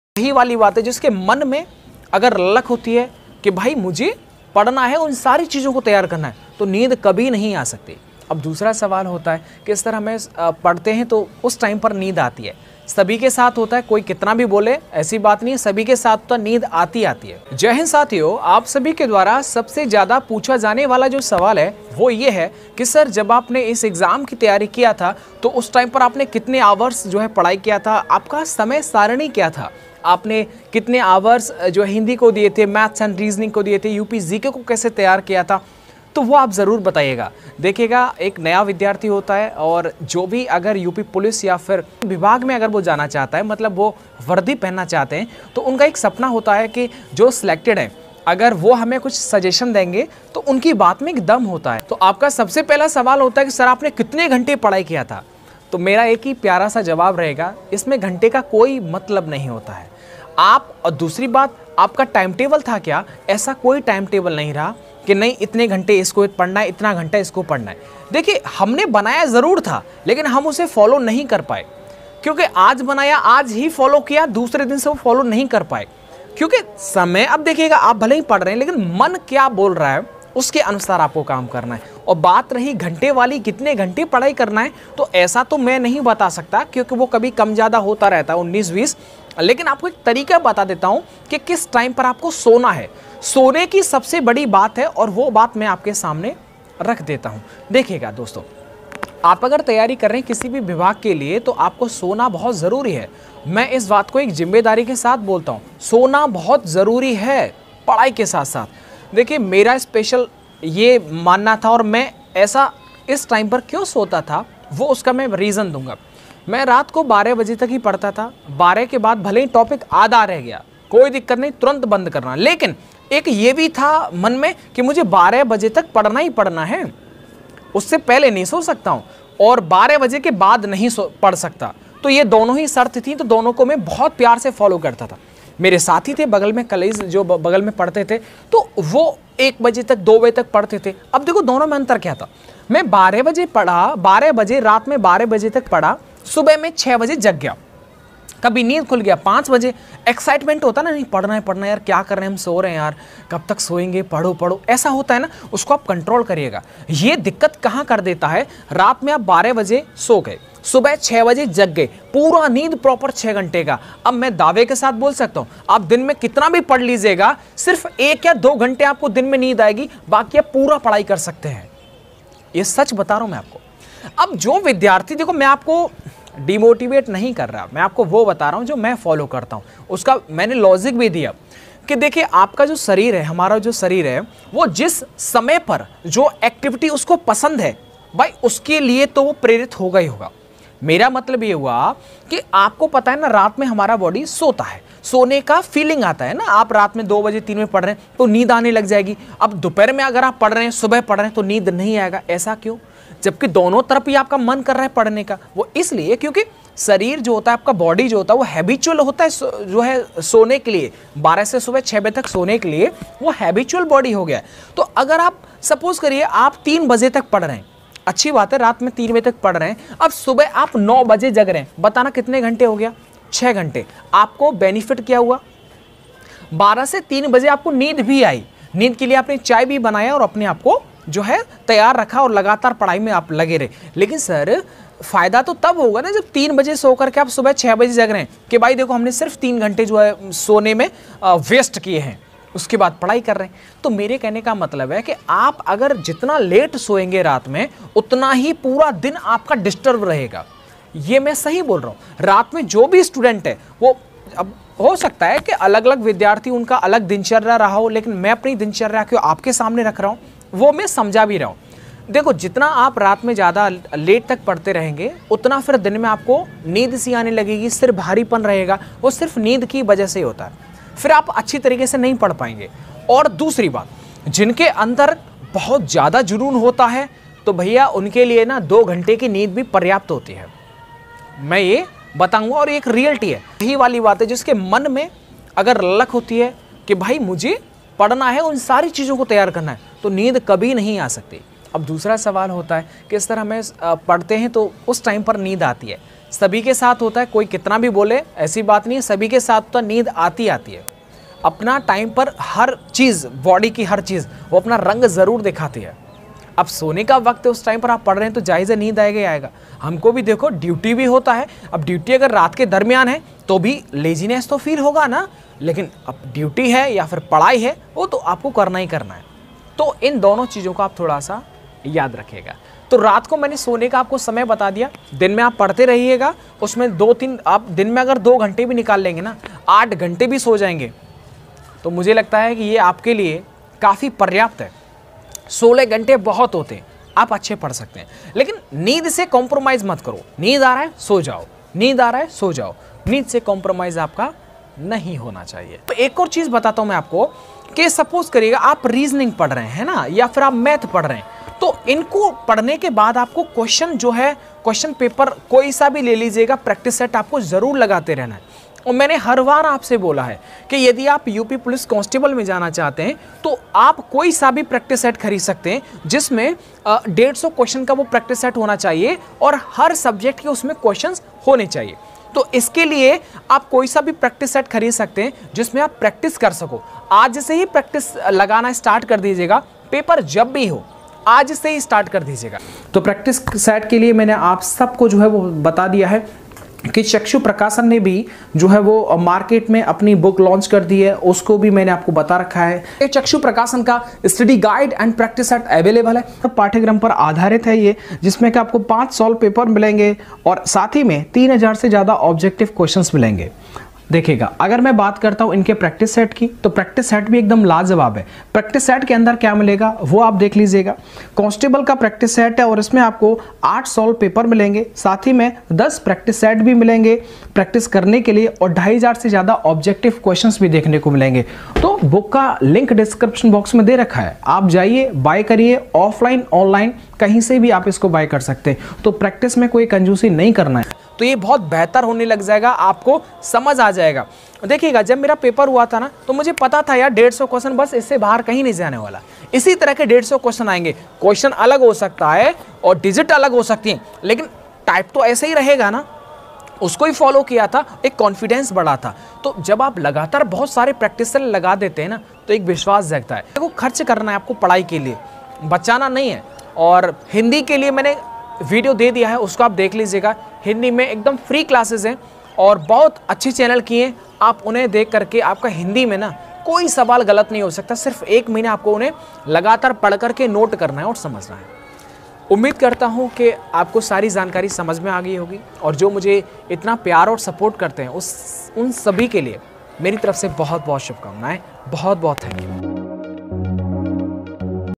मैं कितने घंटे पढ़ता था । कितने बजे सोना था जानकर चौक जायेंगे आप UP POLICE CONSTABLE 60244
UP POLICE CONSTABLE Best BOOK 2023-24
COMBO PACK
PRACTISE SET
FLIPKART